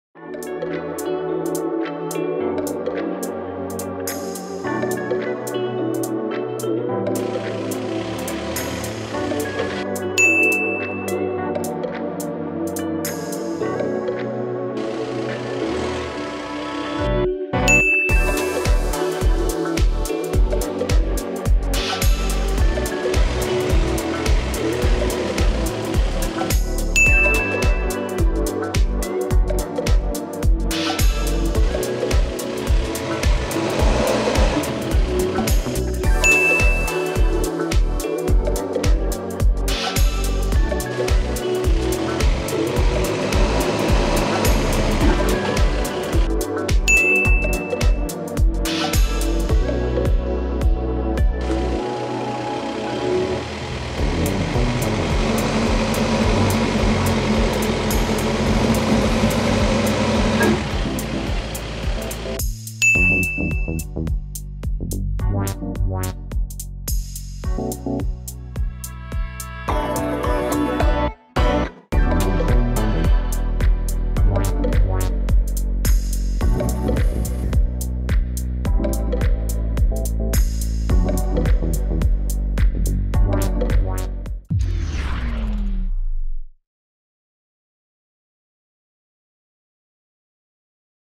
Škoda Superb 4x4 III 4x4 Test on Rollers - CarCaine
Škoda Superb 4x4 III generation tested on rollers. How many stars can it get in 4x4 Test on Rollers?